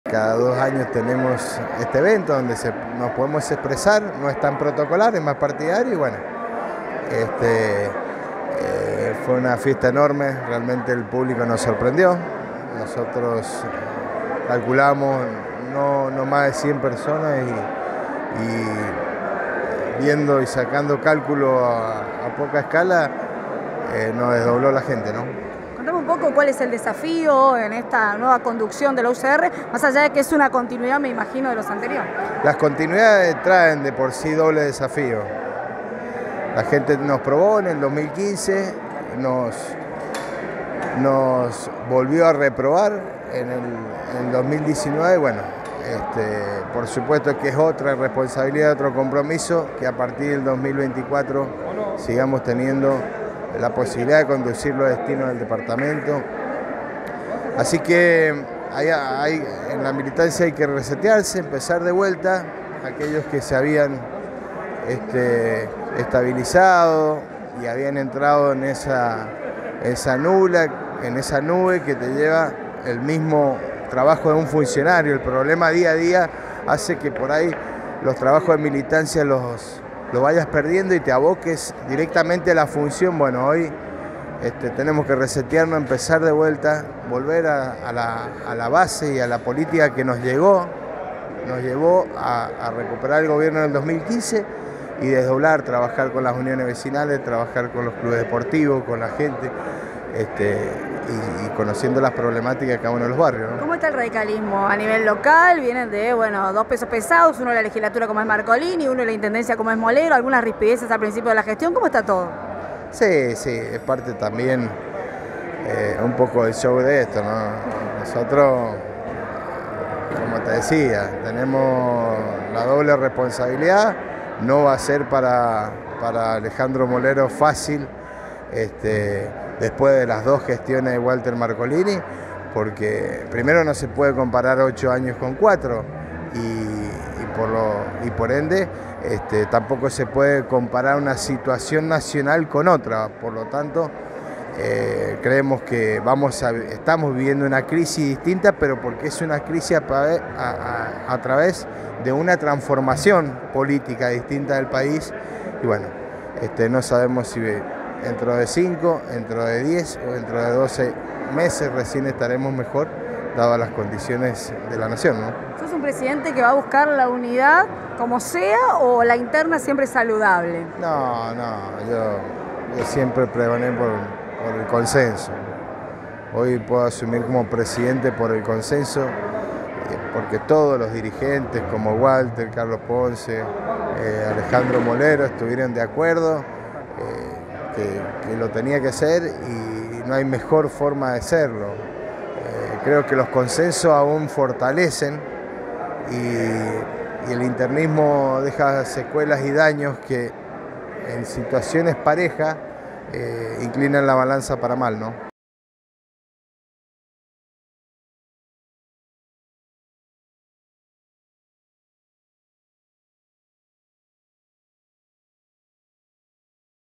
LEONARDO VIÑOLO- PTE. UCR “EN LA MILITANCIA HAY QUE RECETEARSE”
El ex senador y actual consejero de Vialidad Provincial en la zona Sur, Leonardo Viñolo, asumió la presidencia de la UCR en el departamento con un fuerte mensaje interno de “volver a las bases” y a la forma de hacer política que los llevó a recuperar la Intendencia del departamento en 2015.